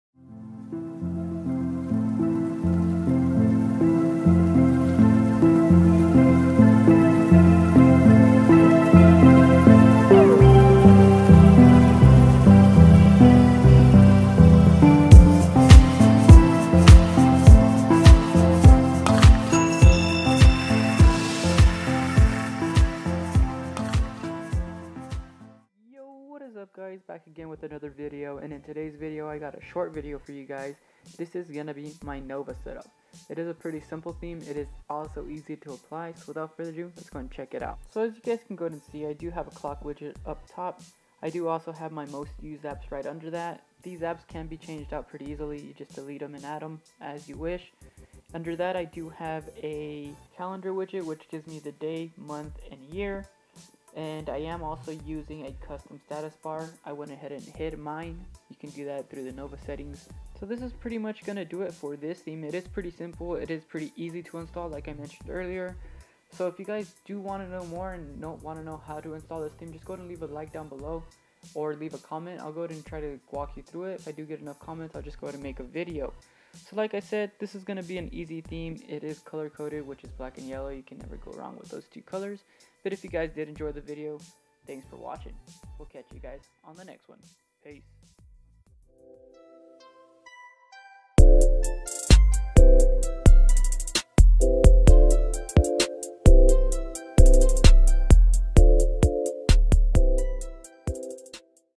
Best Nova Launcher Setup (2018) Nova Theme
Best Nova Launcher Setup (2018) Best Nova Setup Theme is really easy to install all you will need is the wallpaper a custom icon pack and zooper widget. This nova Setup is unique and is themed around the DarkNight (Batman). this is one of the best android home screen step, you would need to download nova launcher to get the best android nova launcher setup.